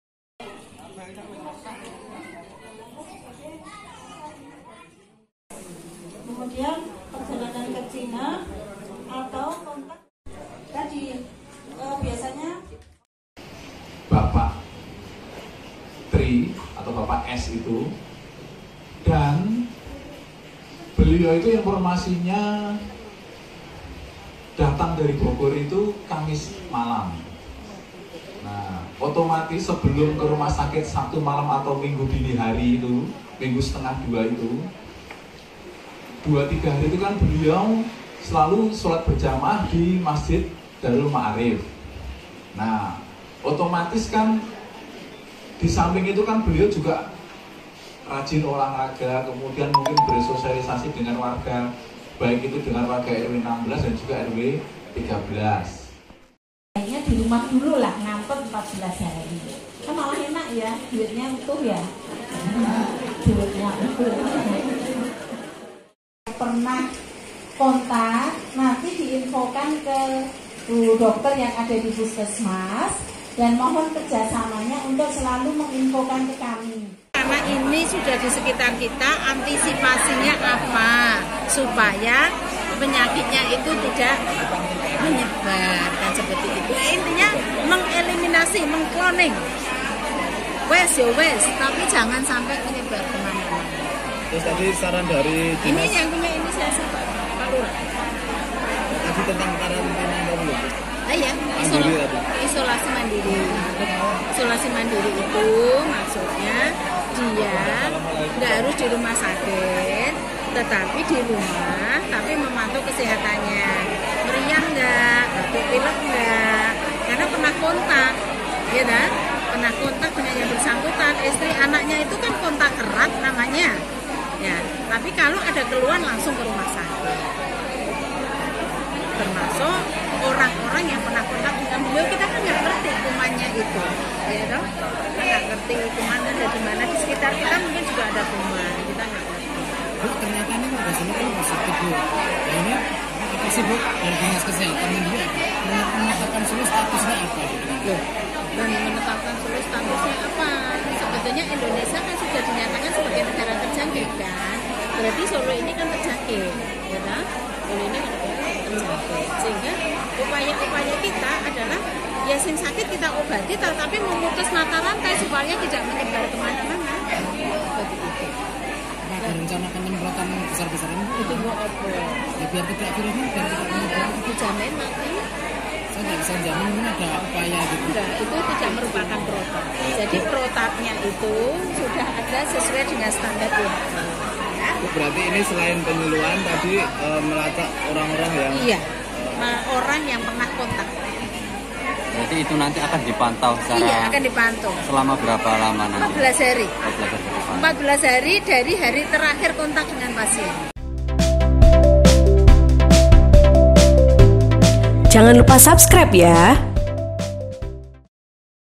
Perilaku Hidup Bersih dan Sehat Cegah Virus Corona
Selengkapnya Dinas Kesehatan (Dinkes) Solo akan mendata dan memantau warga sekitar tempat tinggal pasien meninggal dunia yang dinyatakan positif corona atau covid-19. Perlu diketahui sebelumnya, setelah mendapatkan perawatan di ruang isolasi RSUD Dr Moewardi Solo, Pasien Dalam Pengawasan (PDP) corona tersebut meninggal dunia pada Rabu (11/3/2020) lalu.